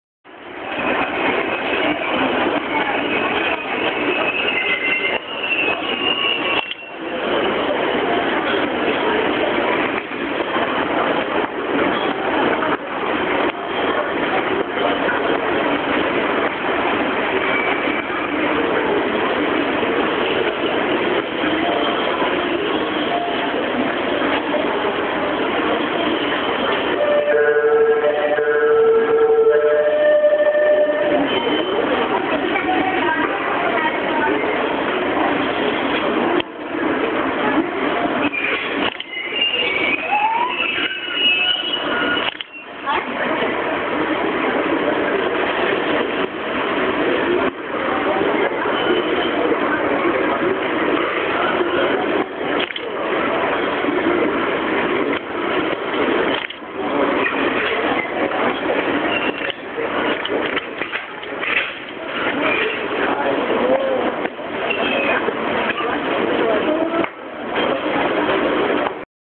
JR 成田エクスプレス連結作業
東京駅にて行われた成田エクスプレス41号の連結作業です。
携帯で録ったため、画質が悪くピントが全然あっていません。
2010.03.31